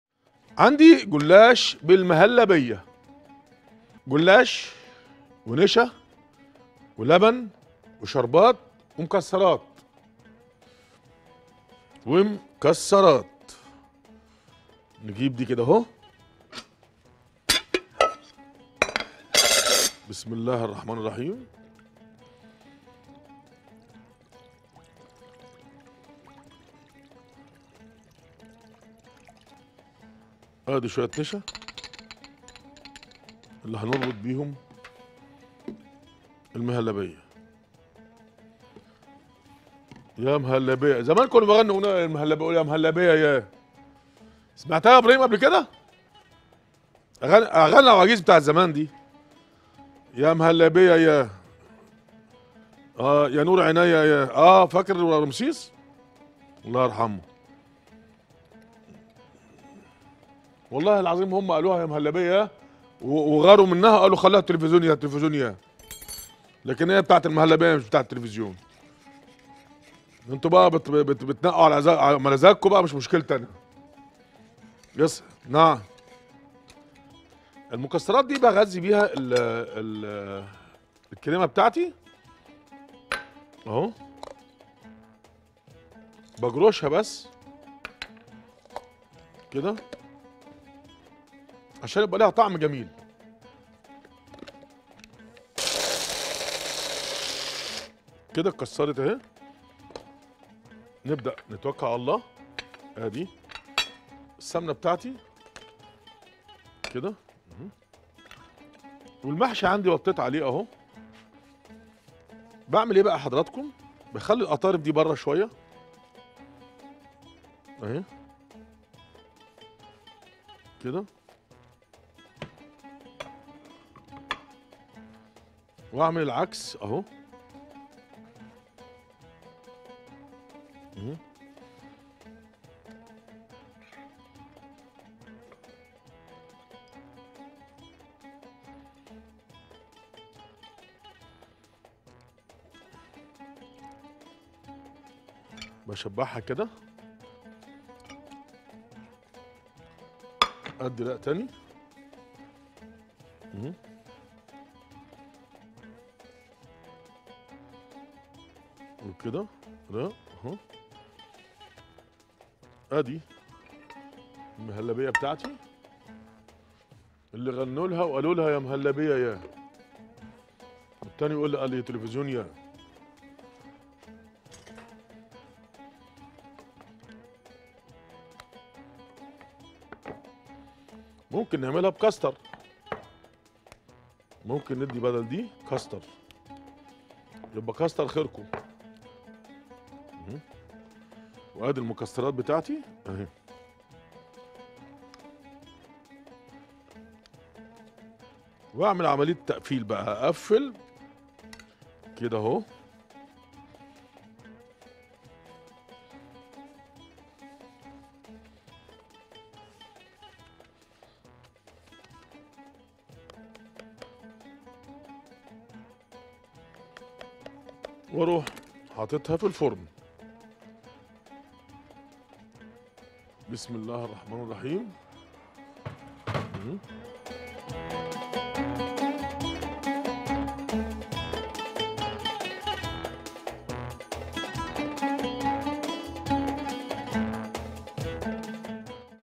جلاش بالمهلبية | الشيف شربيني
طريقة عمل جلاش بالمهلبية مع الشيف شربيني في الشيف